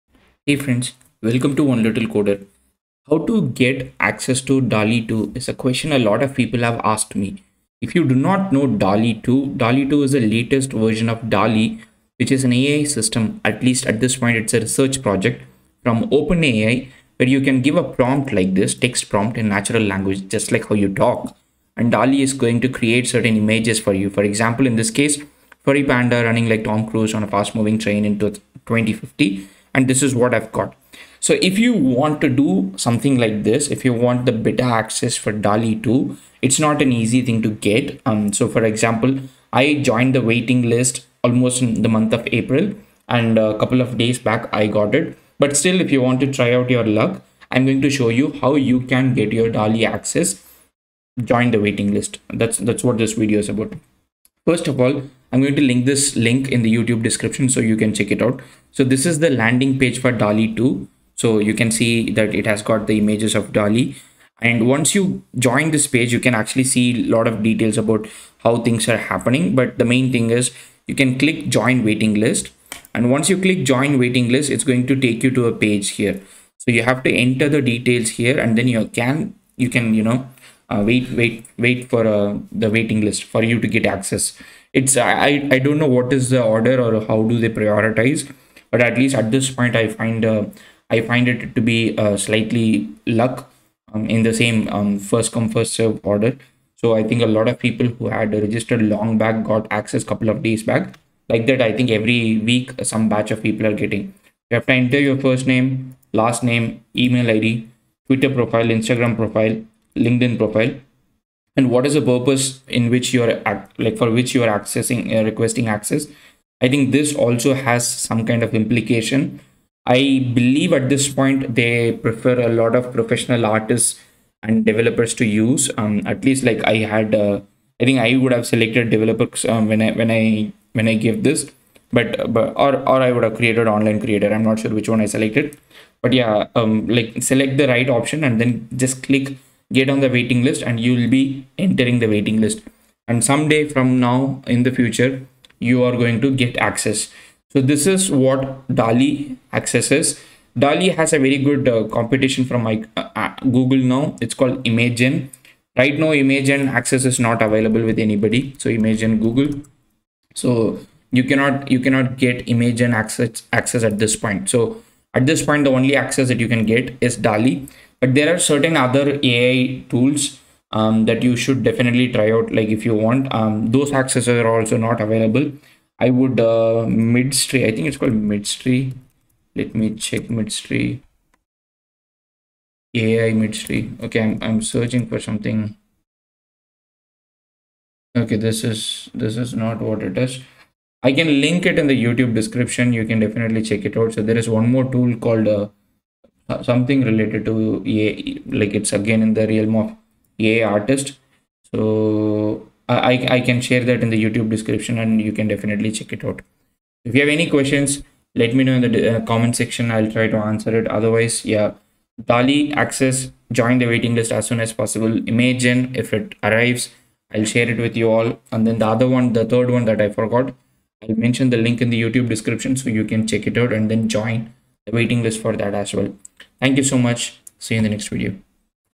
How to get DALL-E 2 Access? | Join DALL·E 2 Waiting List
In this very short video, You'll learn how to get Dall-e 2 access. At this point, it's not open to everyone, you need to join the waiting list and request access.